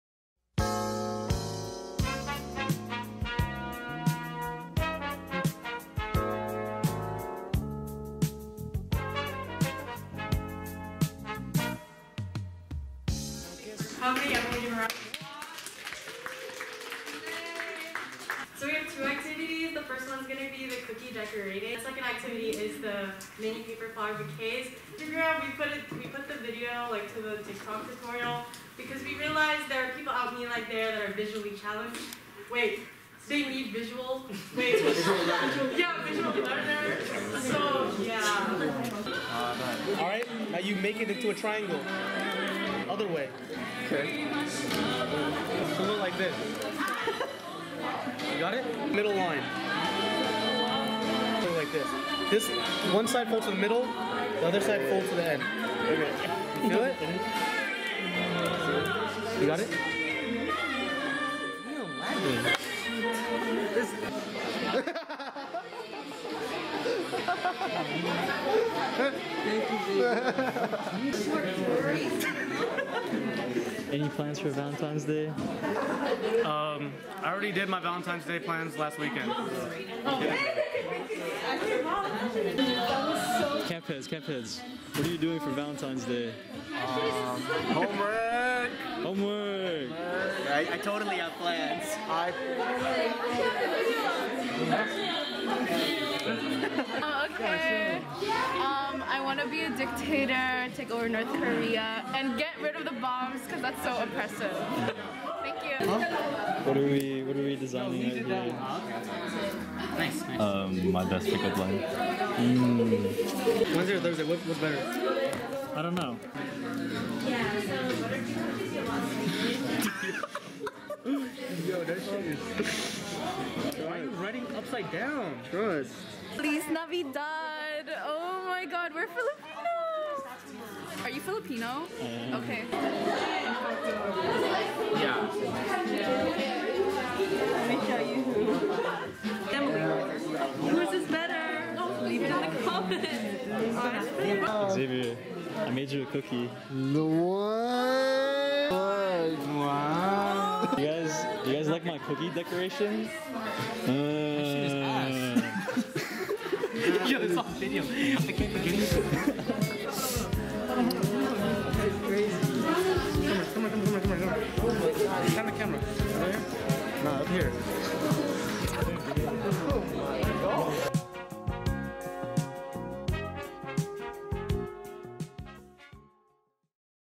Recap | KAMP x Alay Valentine's Event
KAMP and Alay hosted a fun Valentine's Day Event where attendees got to make paper bouquets and decorate heart-shaped cookies!

Filmed & Edited by: Amos Cao